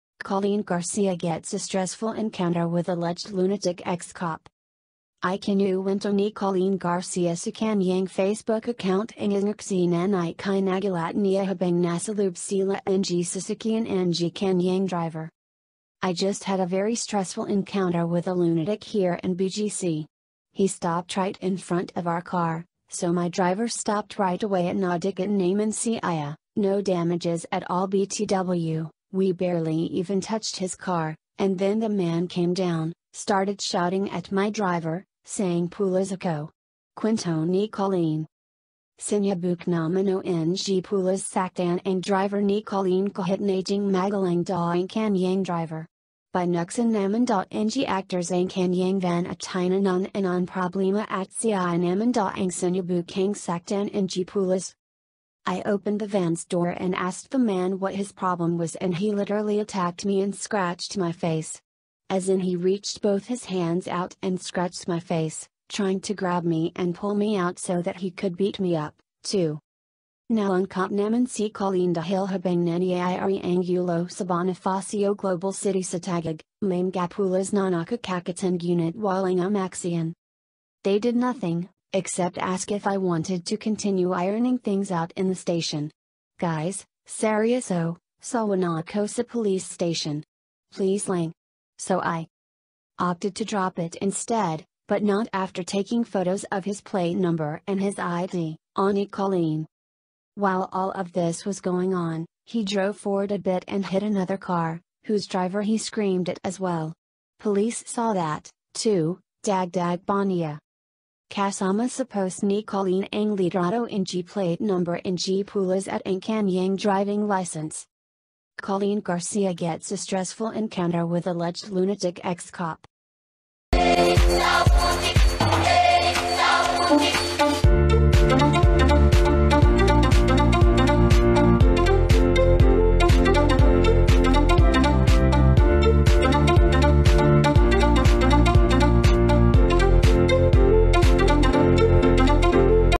Coleen Garcia gets a “stressful encounter” with alleged “lunatic” ex-cop
Philippines News today : Entertaiment scandals

Ikinuwento ni Coleen Garcia sa kanyang Facebook account ang isang eksenang ikinagulat niya habang nasa loob sila ng sasakyan ng kanyang driver.
“I just had a very stressful encounter with a lunatic here in BGC. He stopped right in front of our car, so my driver stopped right away at nadikitan namin siya (no damages at all btw, we barely even touched his car) and then the man came down, started shouting at my driver, saying "PULIS AKO!!!"” kwento ni Coleen.
Sinubukan umano ng pulis saktan ang driver ni Coleen kahit naging magalang daw ang kanyang driver. Binuksan naman daw ng aktres ang kanyang van at tinanong anong problema at siya naman daw ang sinubukang saktan ng pulis.
“I opened the van's door and asked the man what his problem was and he literally attacked me and SCRATCHED MY FACE. As in he reached both his hands out and scratched my face, trying to grab me and pull me out so that he could beat me up, too!”
Nalungkot naman si Coleen dahil habang nangyayari ang gulo sa Bonifacio Global City sa Taguig, may mga pulis na nakakakita ngunit walang umaaksyon.
“They did nothing, except ask if I wanted to continue ironing things out in the station. Guys, seryoso, sawa na ako sa police station. Please lang. So I
opted to drop it instead, but not after taking photos of his plate number and his ID,” ani Coleen.
“While all of this was going on, he drove forward a bit and hit ANOTHER car, whose driver he screamed at as well! Police saw that, too,” dagdag pa niya.
Kasama sa post ni Coleen ang litrato ng plate number ng pulis at ang kanyang driving license.